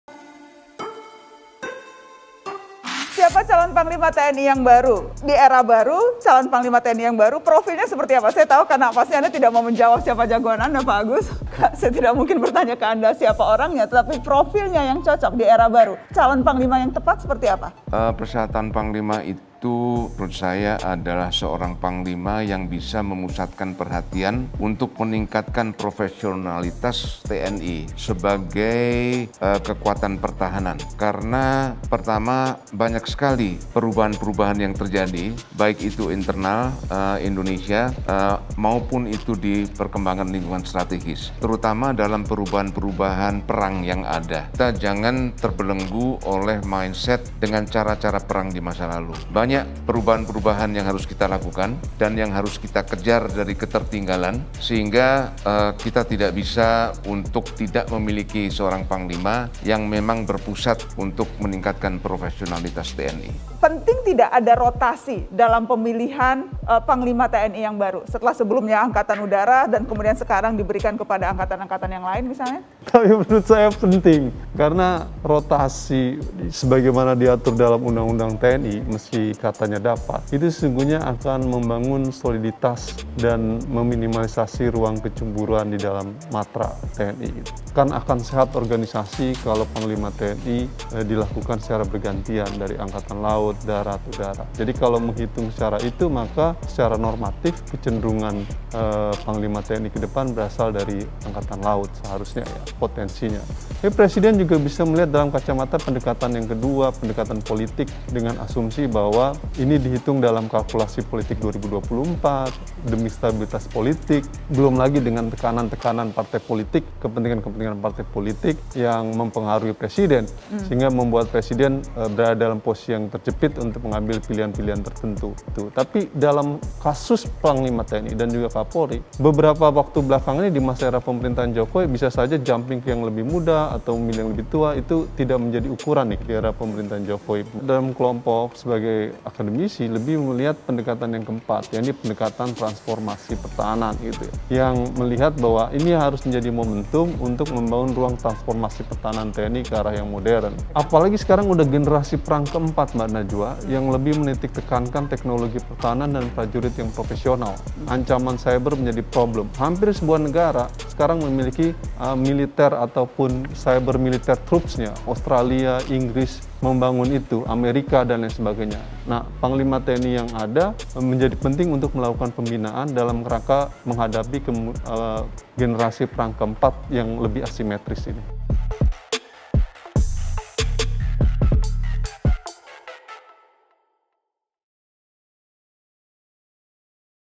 Lemhanas: Calon Panglima TNI Harus Siap Hadapi Perang Cyber | Narasi Newsroom
SIAPA YANG COCOK JADI PANGLIMA TNI PERIODE SELANJUTNYA?

Panglima Tentara Nasional Indonesia (TNI), Marsekal Hadi Tjahjanto akan memasuki masa pensiun pada akhir November 2021 nanti.

Namun, informasi soal nama calon pengganti Panglima TNI belum ditetapkan atau diajukan oleh Presiden Joko Widodo (Jokowi) kepada DPR RI.

Hmm… Kira-kira di zaman seperti sekarang, Panglima TNI yang cocok untuk periode selanjutnya yang kayak gimana, yaa?

(Narasi)